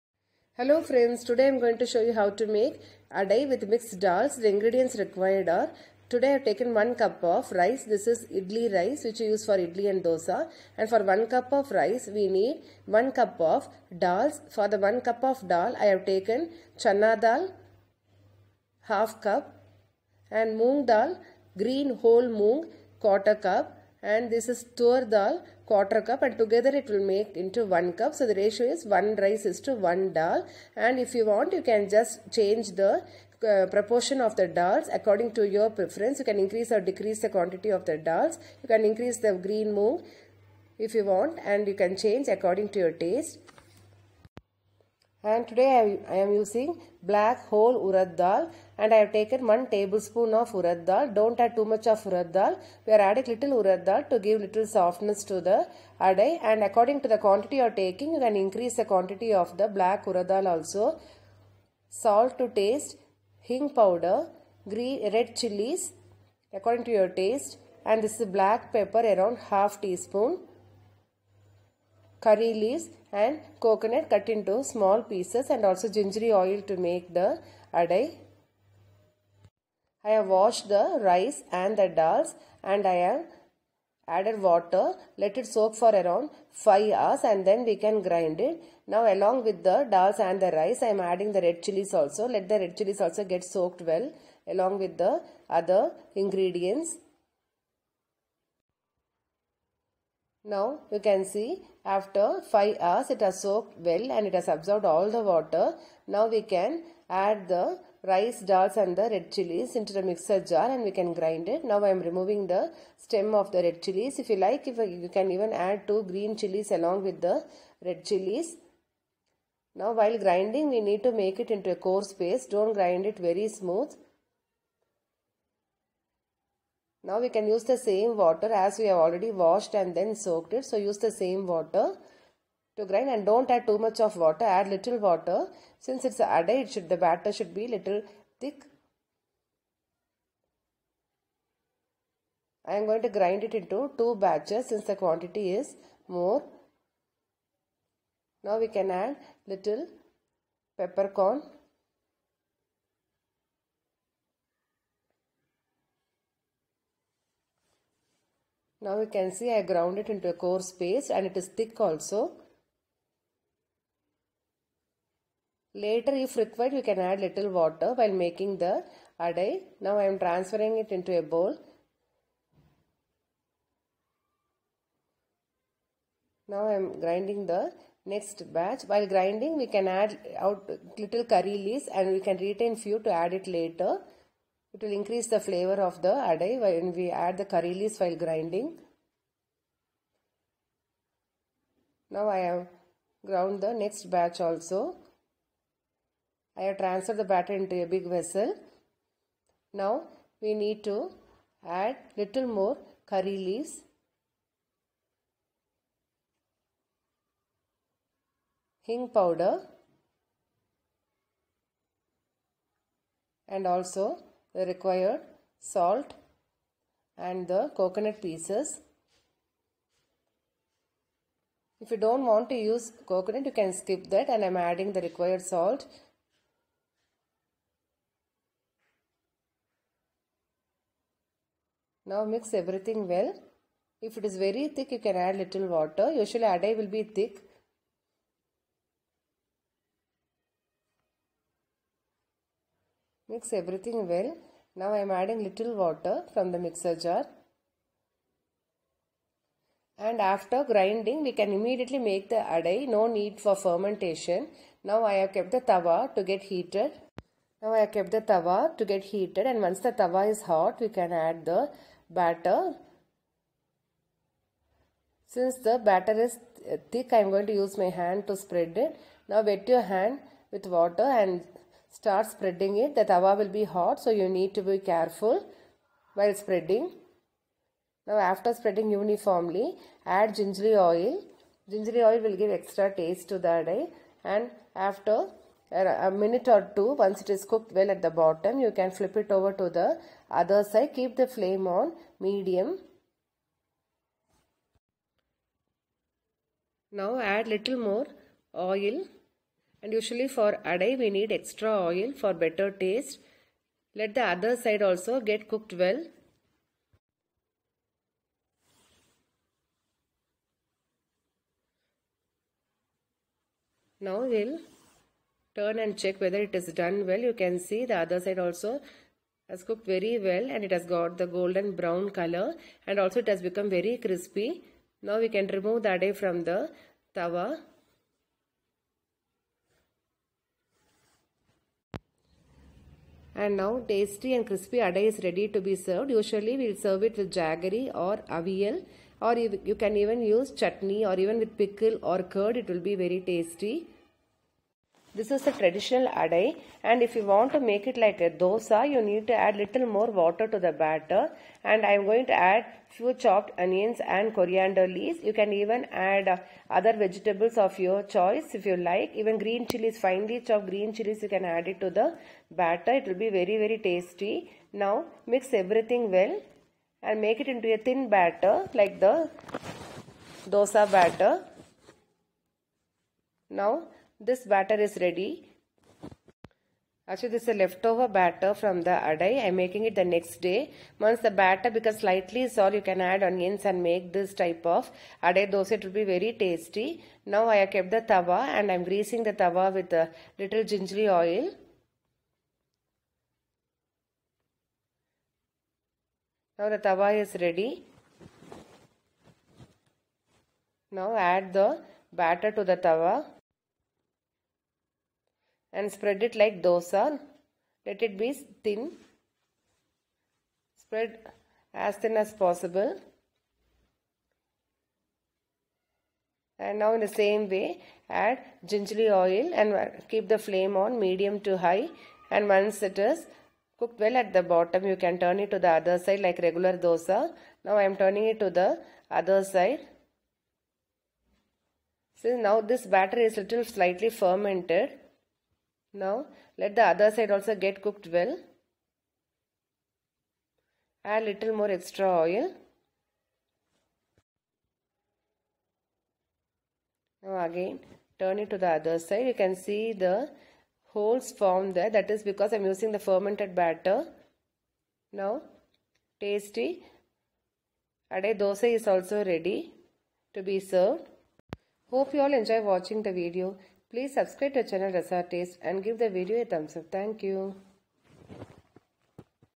Adai recipe
Can be made traditionally as well with onions also like dosa.